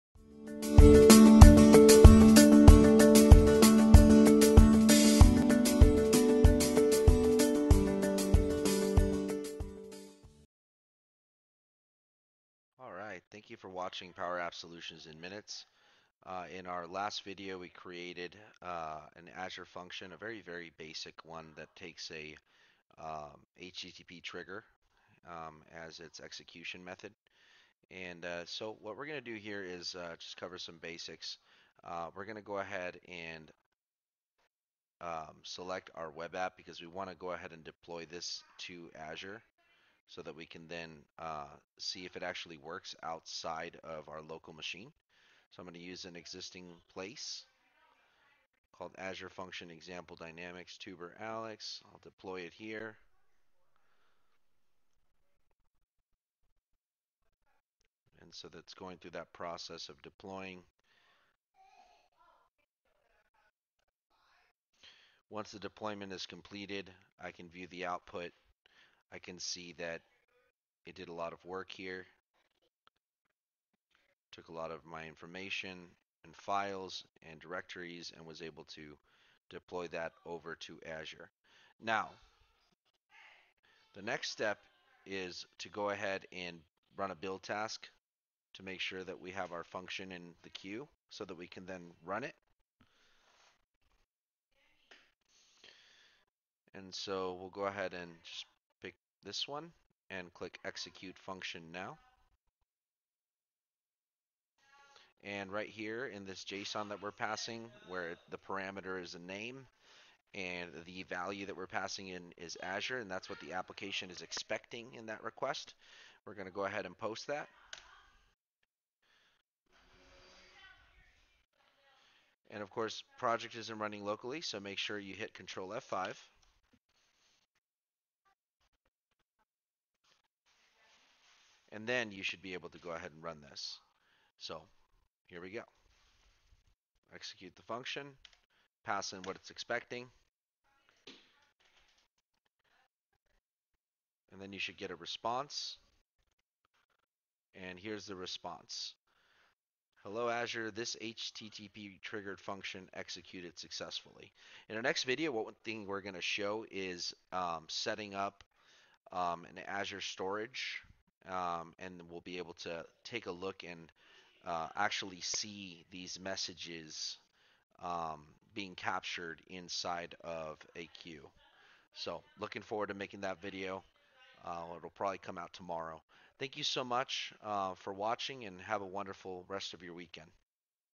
Azure Functions Lesson 2: Deploy your Function to Azure Cloud and Test it!
Leverage the integration between Visual Studio Code and Azure Cloud to deploy your app, and execute your function's logic in the cloud.